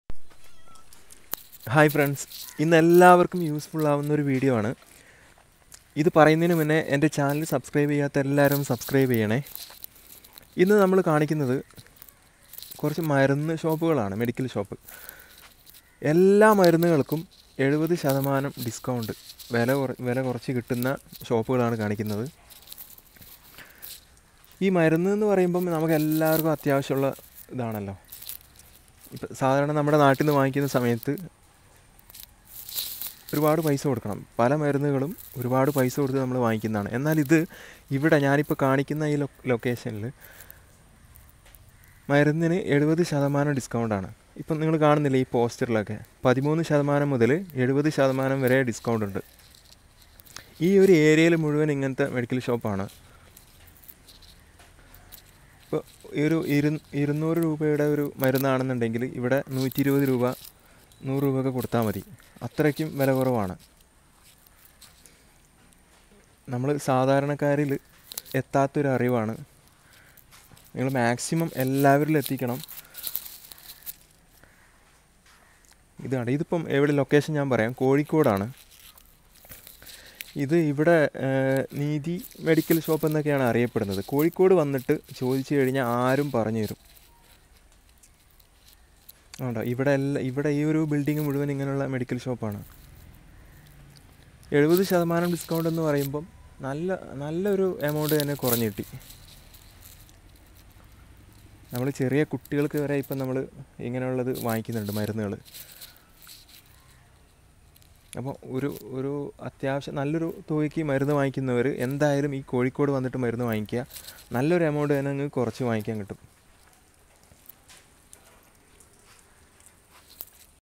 ഇവിടെ മരുന്നുകള്‍ പകുതി വിലയ്ക്ക് കിട്ടും | Low rate medical shop Calicut
Hi friends this is low rate medical shop, neethi medical shop in calicut district Kerala.